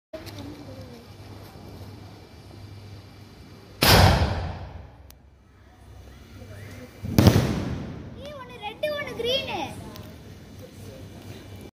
Ananda fireworks-Jack & Jones 3" double ball skyshot-Diwali 2021-Fireworks testing🎇🎆
So today we tested Ananda Fireworks Jack and Jones 3" double ball skyshot which had an amazing red ball and green ball effect!Just try it and have double fun!